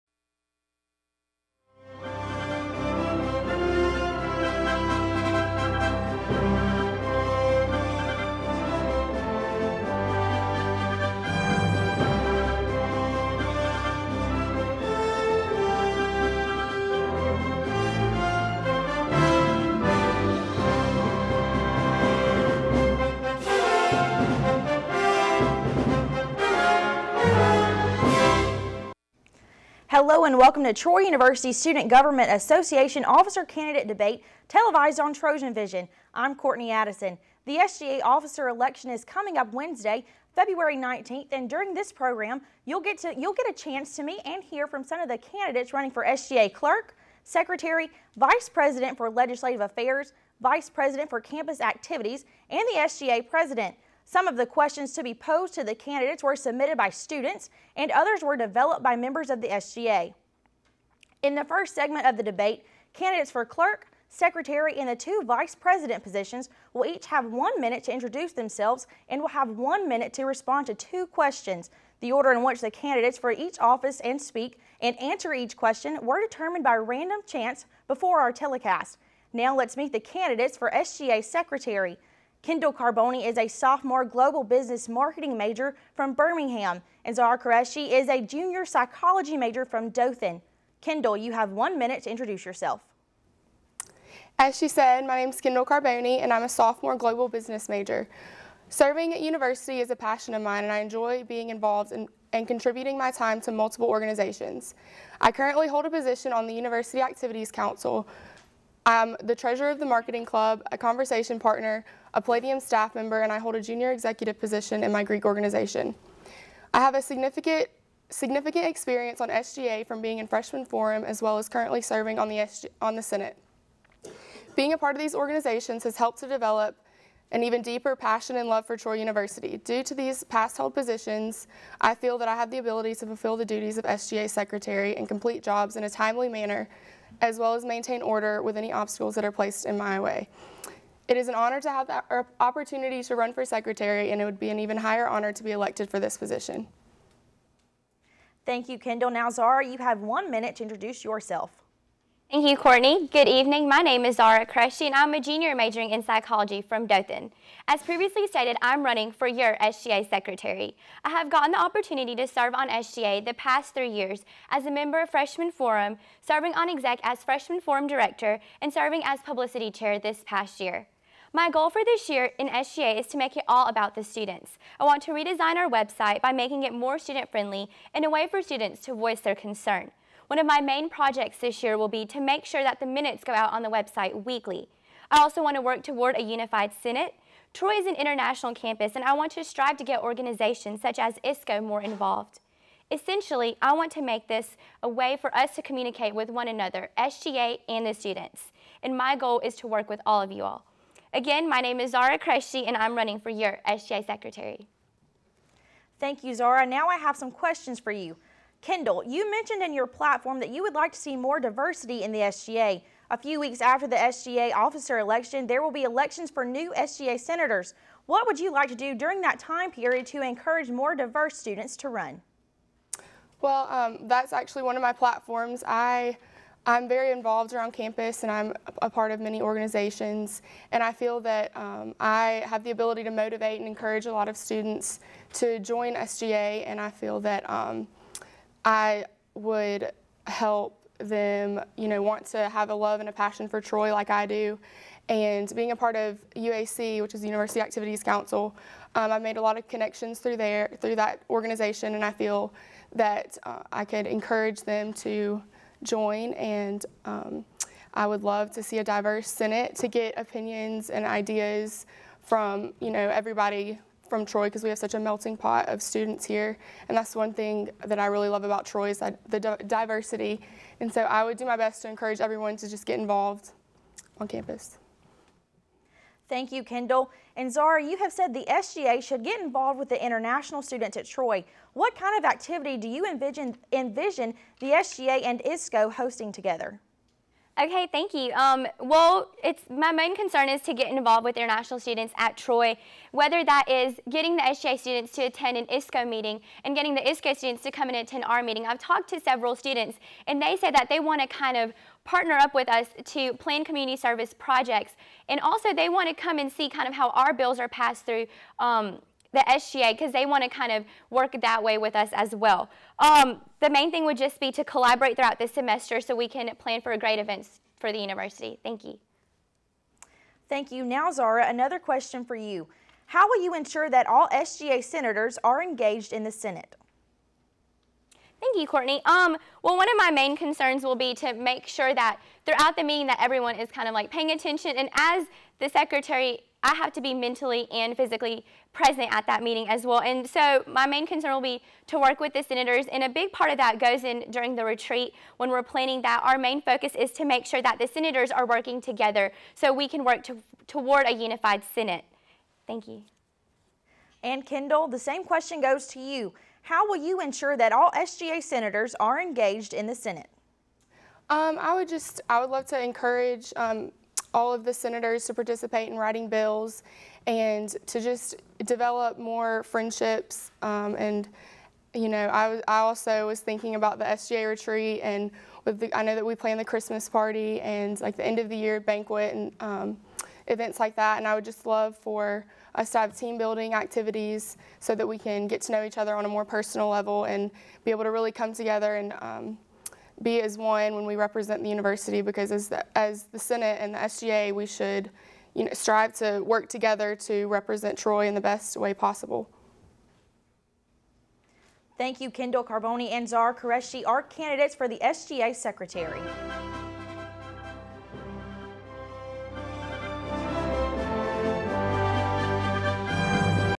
2014 SGA Debate- Secretary
The 2014 Troy University SGA Debate. This segment highlights the candidates for SGA Secretary.